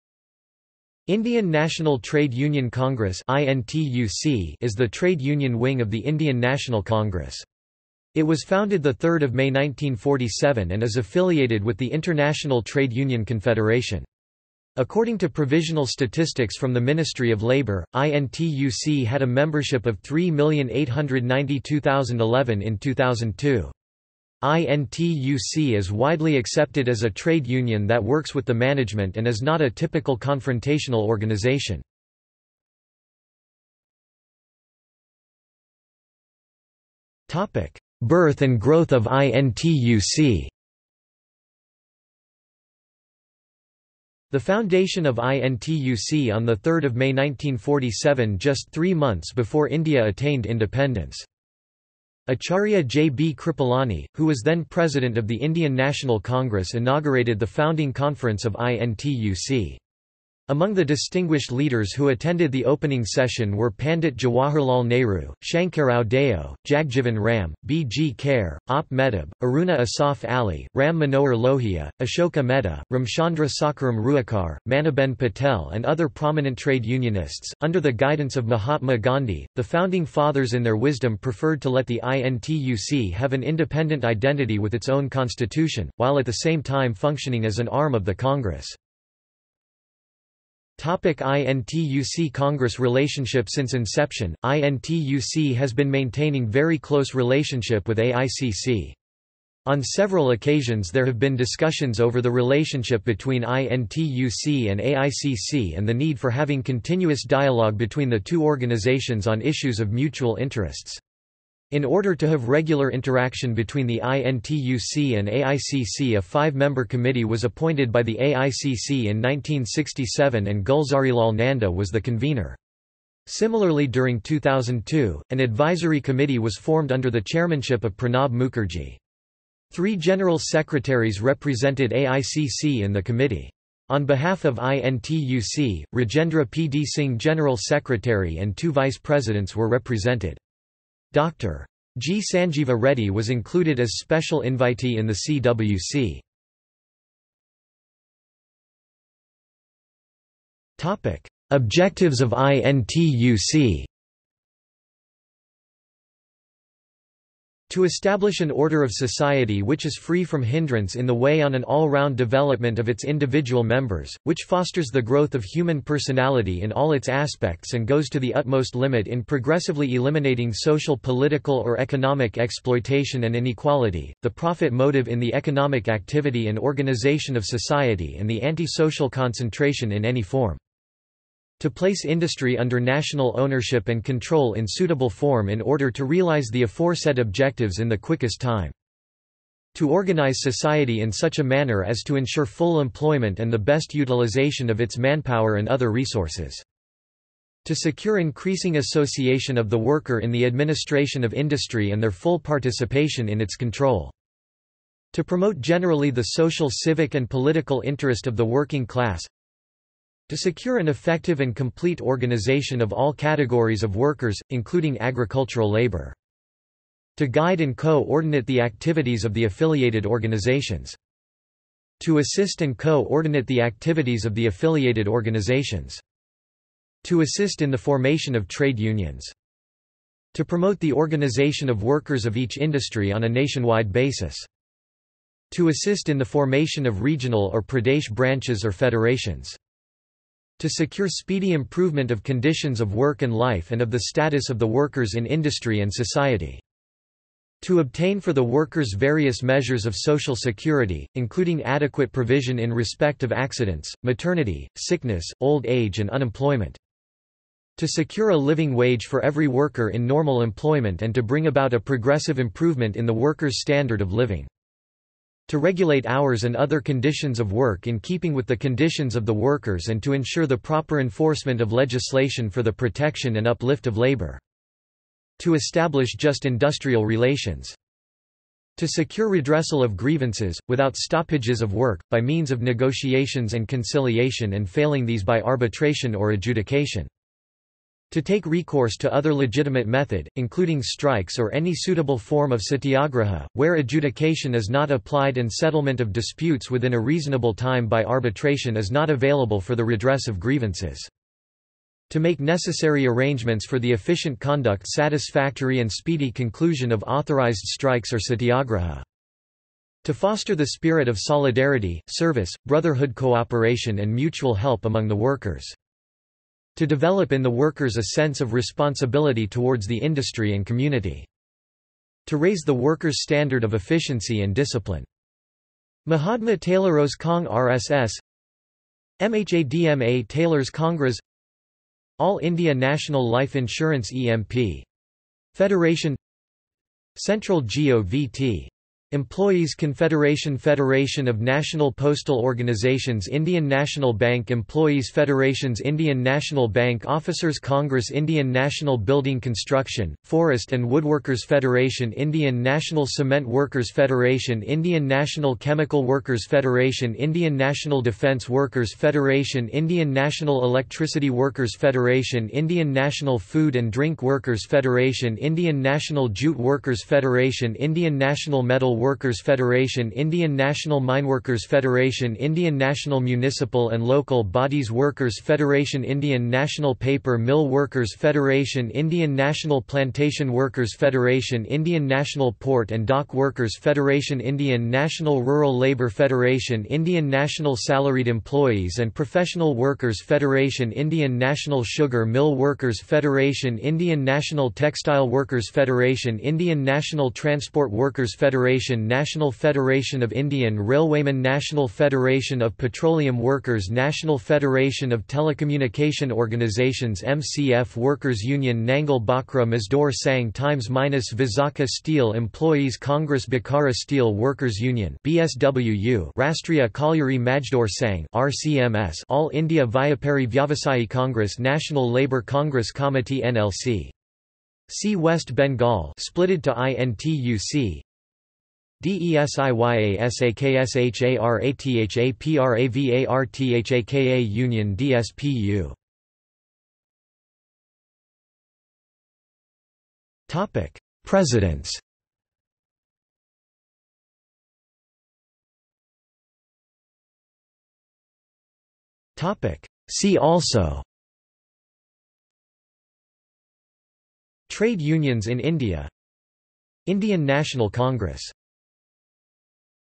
Indian National Trade Union Congress | Wikipedia audio article
This is an audio version of the Wikipedia Article:
Indian National Trade Union Congress

00:00:33 1 Birth and growth of INTUC
00:01:30 2 INTUC Congress relationship
00:02:27 3 Objectives of INTUC
00:07:16 4 Presidents
00:07:25 5 See also

- Socrates

SUMMARY
Indian National Trade Union Congress (INTUC) is the trade union wing of the Indian National Congress. According to provisional statistics from the Ministry of Labour, INTUC had a membership of 3,892,011 in 2002. INTUC is widely accepted as a trade union that works with the management and is not a typical confrontational organisation.